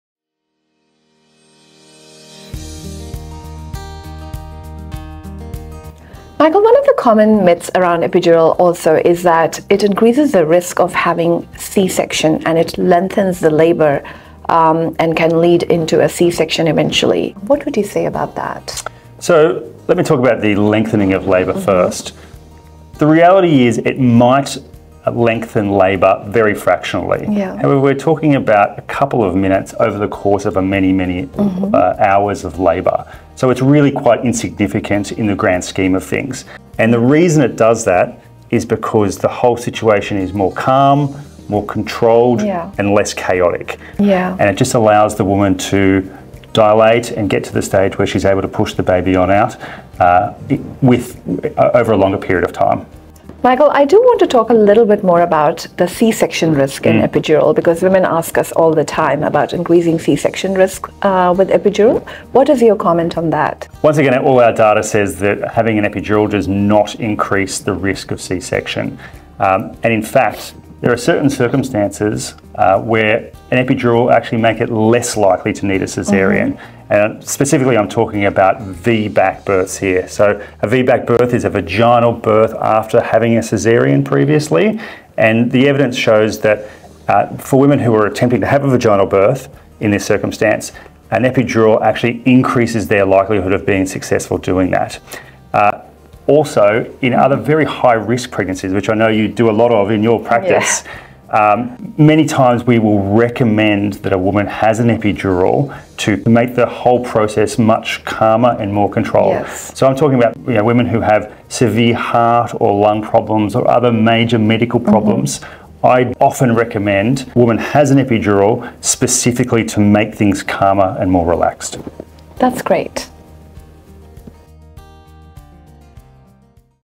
Does epidural increase the risk of C-section?
Worried that an epidural might lead to a longer labour or increase the risk of C-Section?  Westmead high-risk obstetrician & foetal medicine expert Dr Yana Shetty and obstetric anaesthetist Dr Michael Booker answer your most-asked questions about epidural complications in relation to C-section in childbirth. Find out how to relax, breathe and deliver, with no extra risk of a C-Section plot twist!